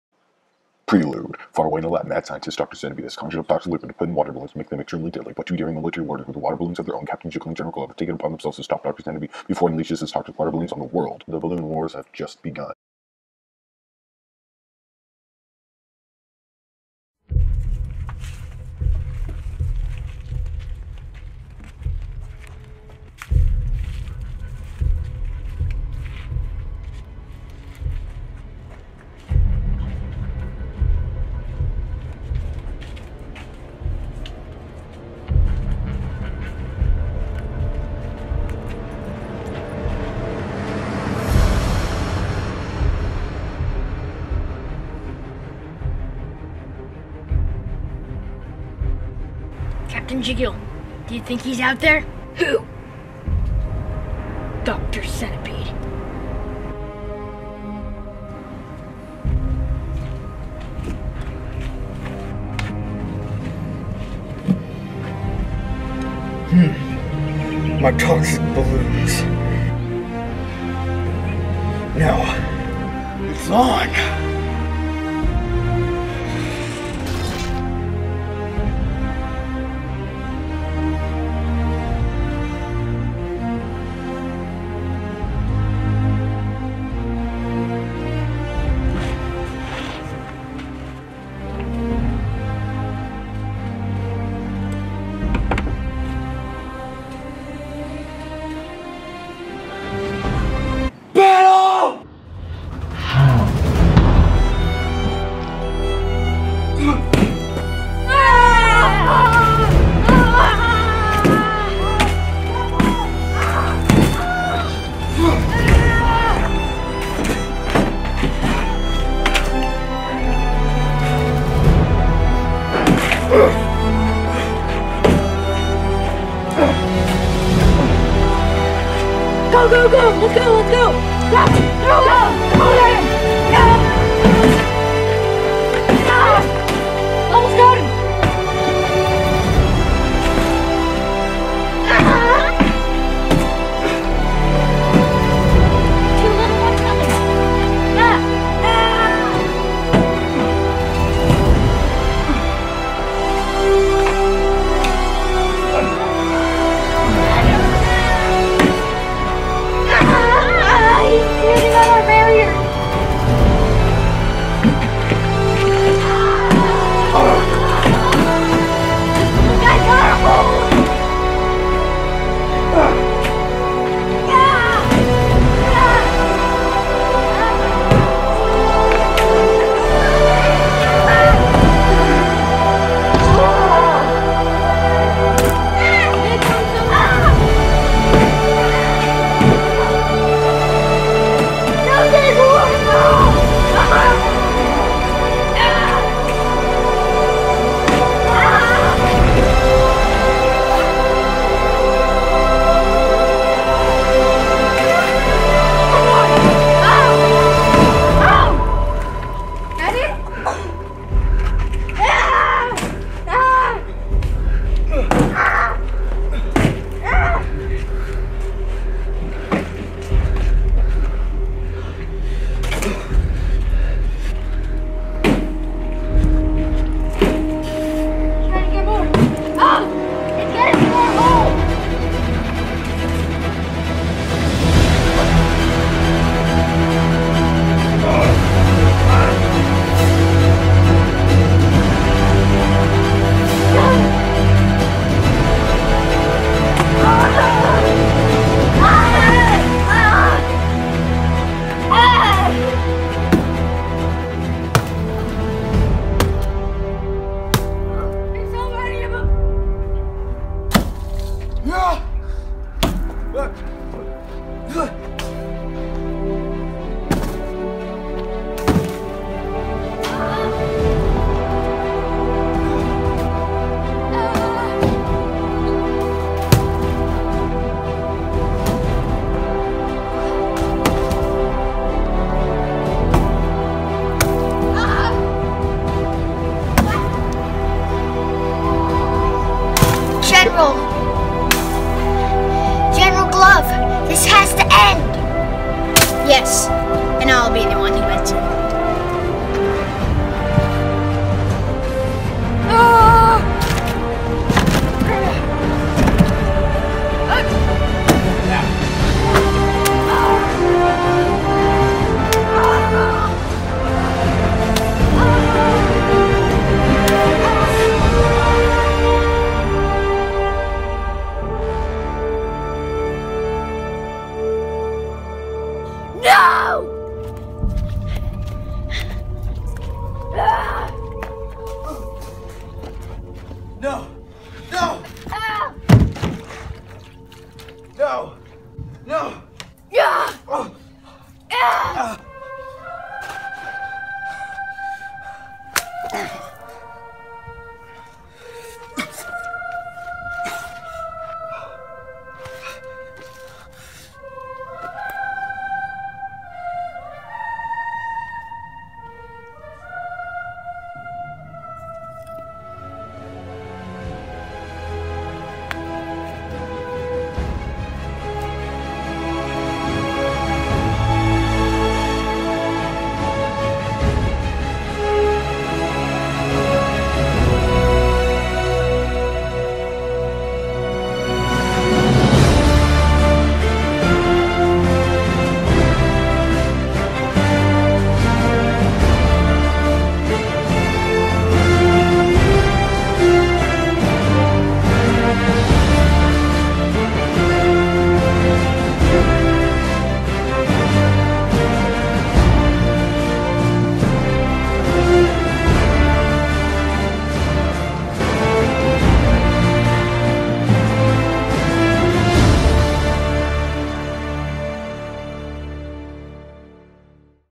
Balloon Wars (OFFICIAL MOVIE) 2017
In a daunting war zone, Dr. Centipede will unleash hundreds of toxic water balloons upon the face of the world. But, two daring military warriors have taken it upon themselves to stop this madness and conquer Dr. Centipedes vicious plan. The Balloon Wars have just begun...

STARRING:
Zoey Rice as Captain Jiggle
Blaze Rice as General Glove
Zander Rice as Dr. Centipede